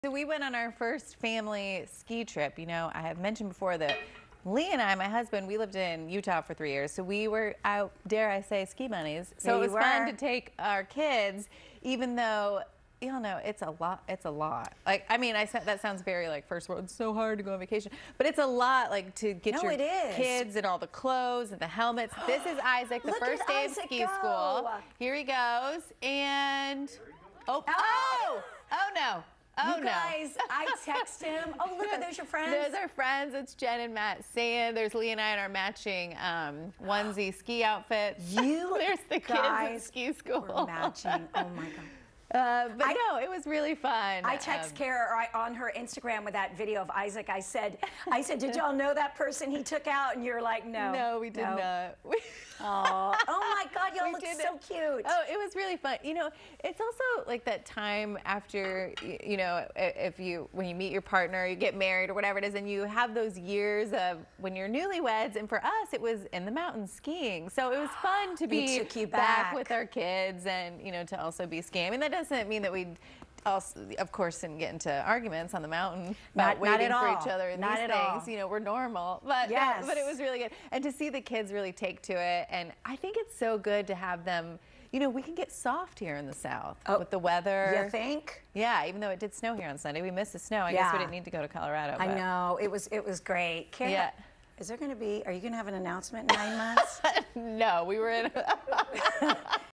The Kneer Family in Colorado
Cara and husband Lee took the kids on their first family ski trip.